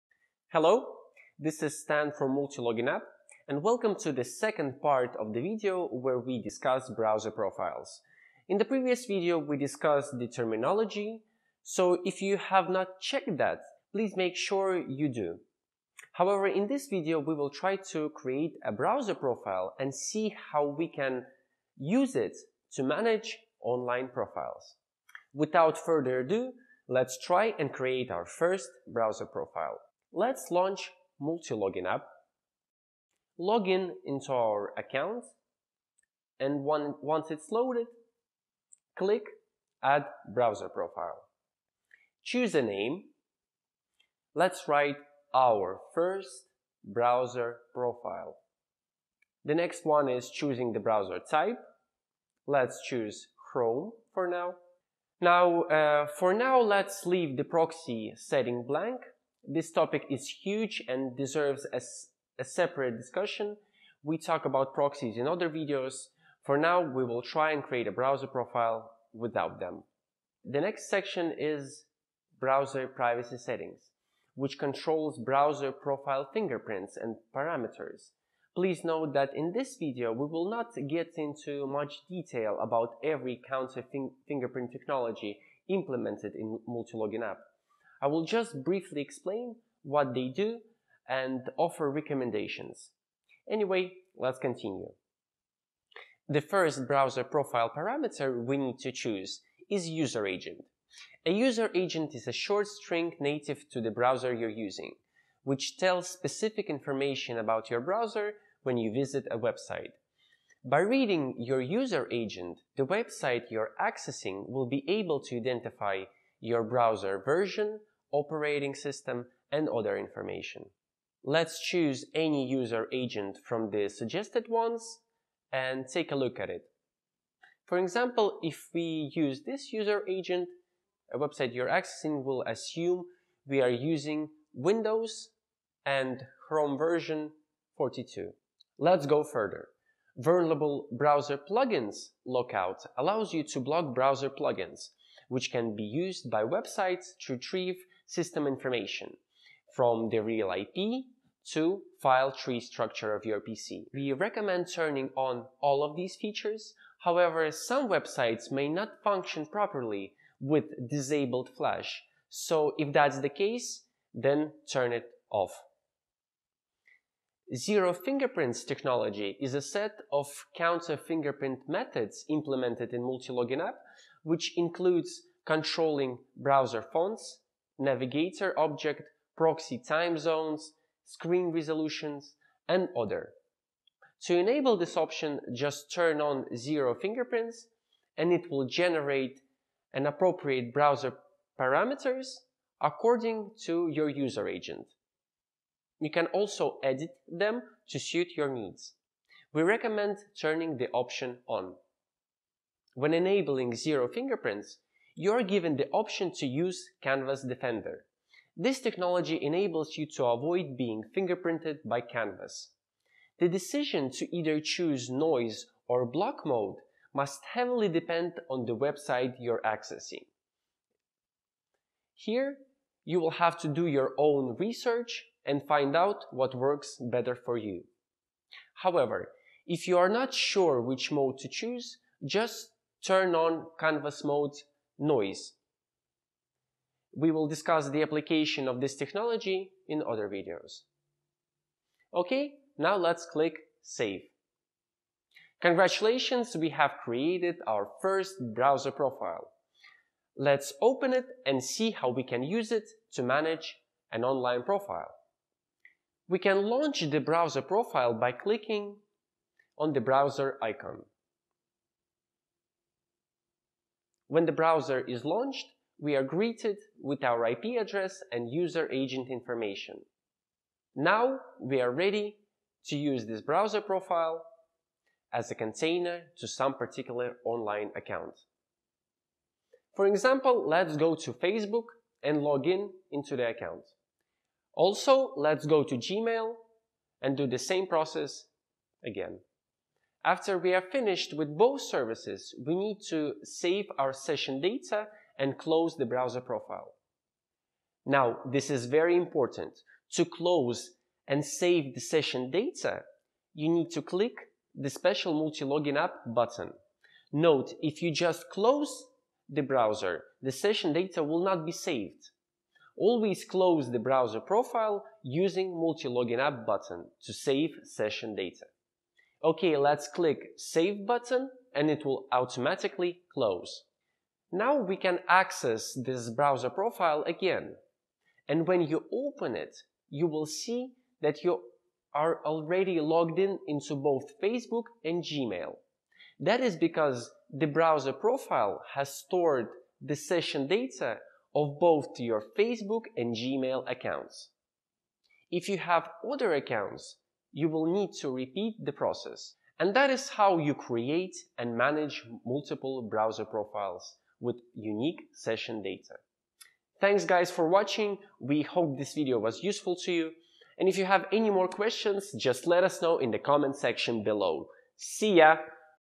Browser profiles Part 2: Creating your first browser profile
This is a second part of the video, where discuss browser profiles. The first part was about the terminology. However, this one is more practical.

In this video we guide you through the whole process of creating a browser profile. We also show how you can utilize browser profile in managing online accounts.

Make sure you watch the first part as well.

Check out Multiloginapp for a cutting-edge online identity management platform.

Let us know if you have any questions in the comment section